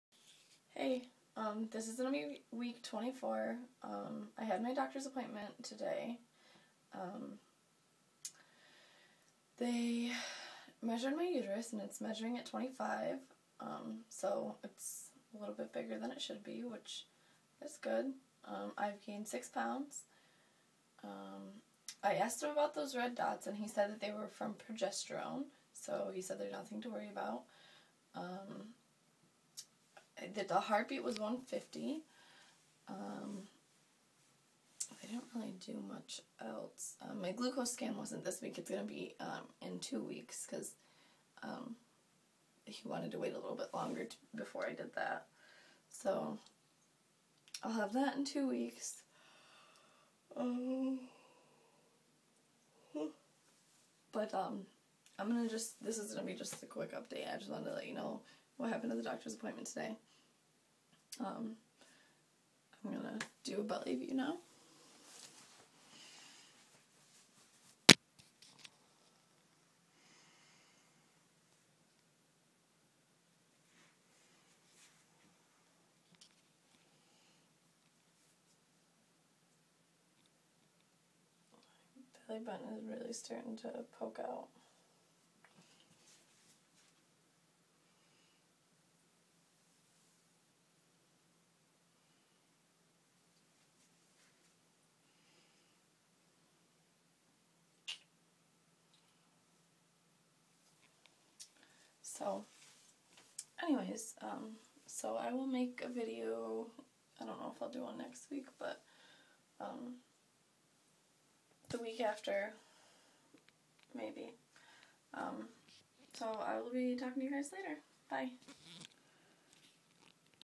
Pregnancy week 24.
Gained 6lbs.
Heartbeat 150.
Uterus is measuring at 25.